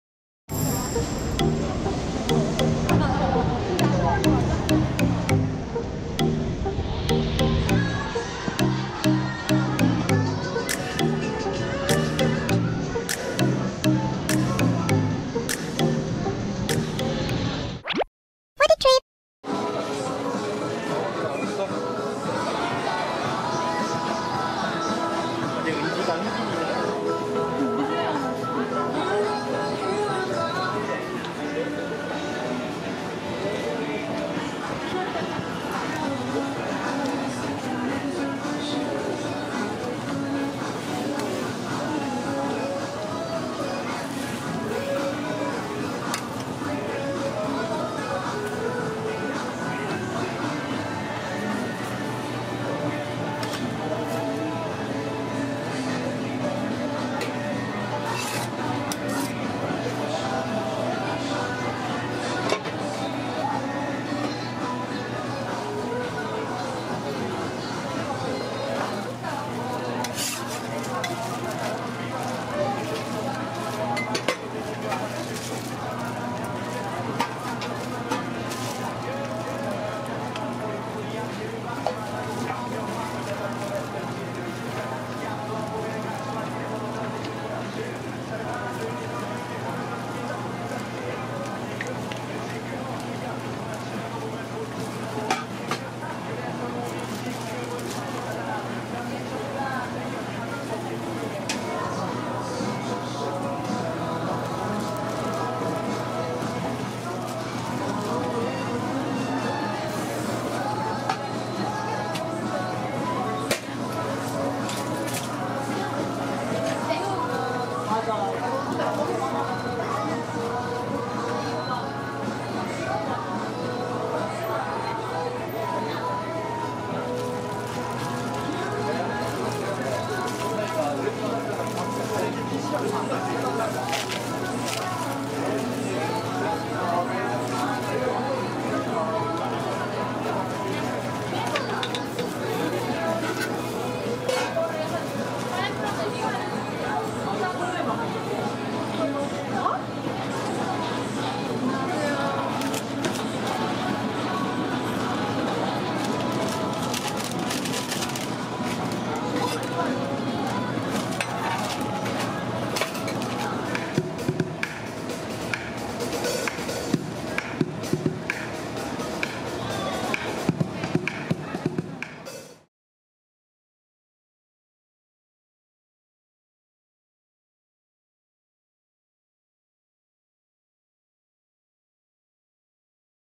delicious Chinese Crepe JianBing (煎餅) - Korean Street Food / Hongdae, Seoul
Hongdae Street Food - JianBing
3,800 KRW (USD 3.37)

Add Toppings : Bacon, Chuibing, Coriander
2,200 KRW (USD 1.95)